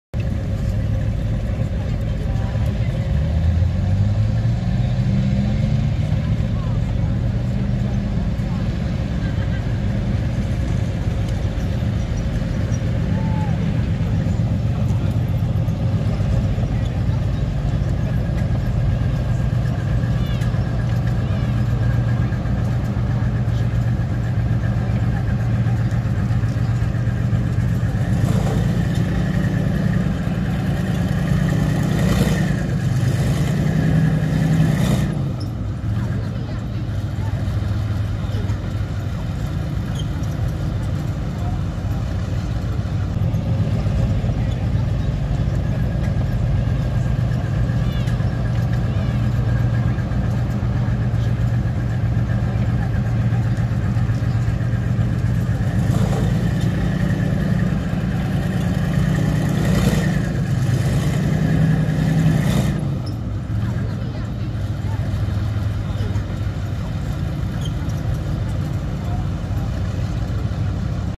Behind the scenes at Bovington Tank fest 2023
A Friend of mine works at Bovington tank museum and took this shot for me on the first day of tankfest, 24th June 2023 of all the tanks and other vehicles waiting to go out.
I asked if he could get a bit closer today so hopefully some more shots later today or tomorrow.